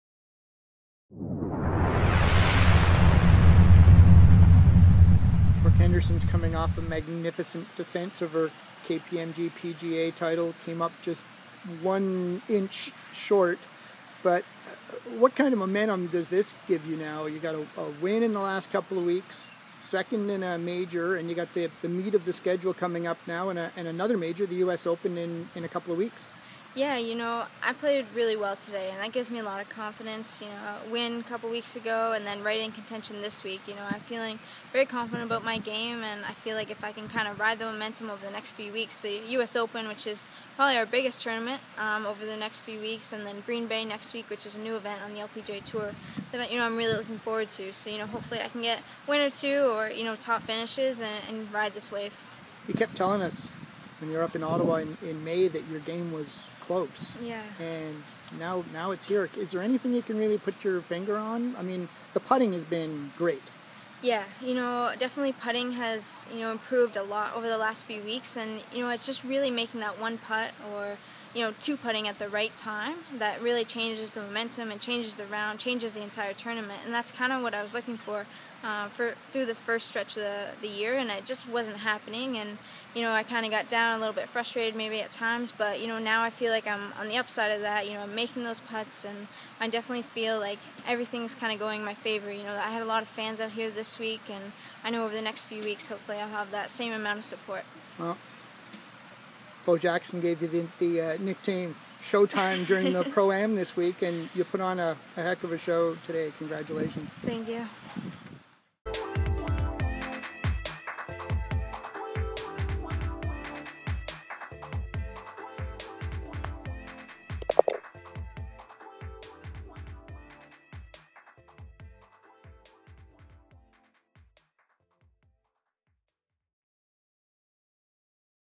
Brooke Henderson KPMG Women's PGA Final Round
Brooke Henderson speaks with Chris Stevenson after the final round of the 2017 KPMG Women's PGA Championship